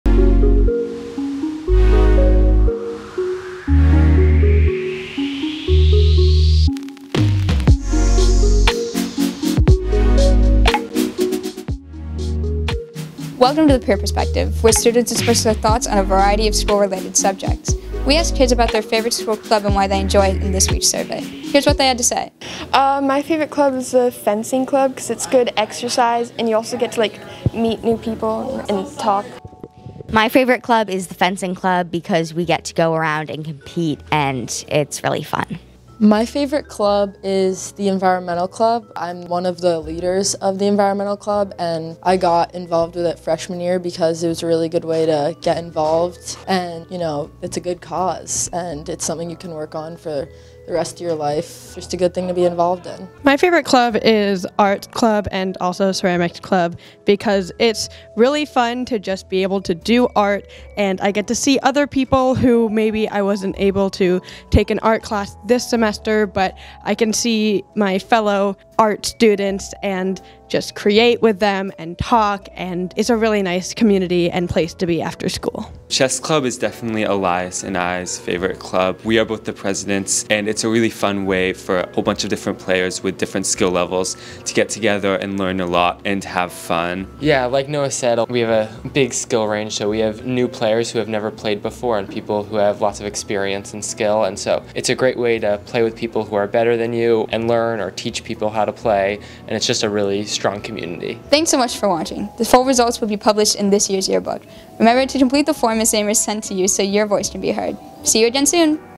Peer Perspective | Favorite Club
Welcome to The Peer Perspective, where students express their thoughts on a variety of school-related subjects
We asked kids about their favorite school club and why they enjoy it in this week's survey. Here's what they had to say: